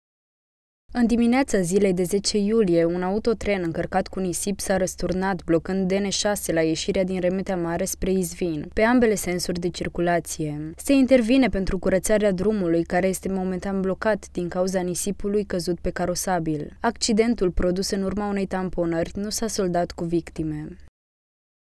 Trafic blocat la Remetea Mare în urma unui accident
În dimineața zilei 10 iulie, un autotren încărcat cu nisip s-a răsturnat, blocând DN 6 la ieșirea din Remetea Mare spre Izvin, pe ambele sensuri de circulație. Se intervine pentru curățarea drumului, care este momentan blocat din cauza nisipului căzut pe carosabil